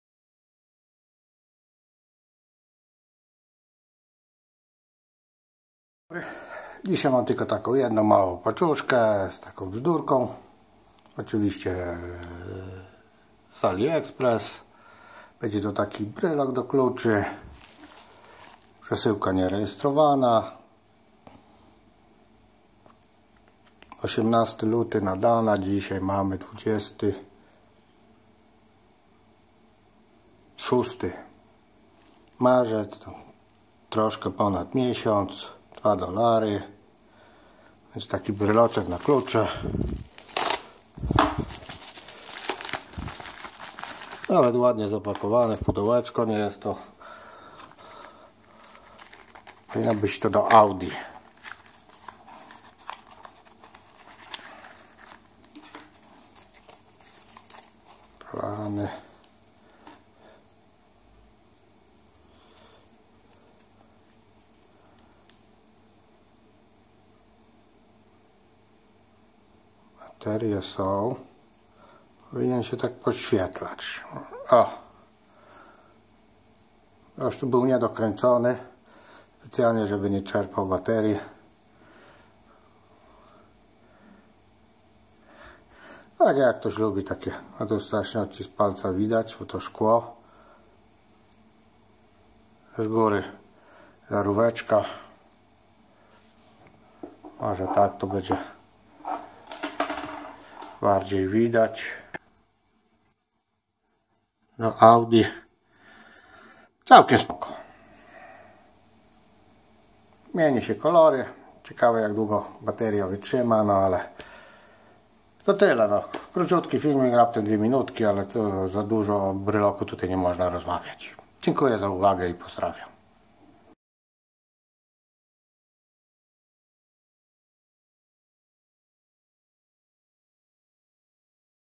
Unboxing. Zakupy na Aliexpress. Paczka z Chin.
Poniżej link do sklepu GearBest. Fajny uchwyt na telefon do samochodu połączony z ładowarką: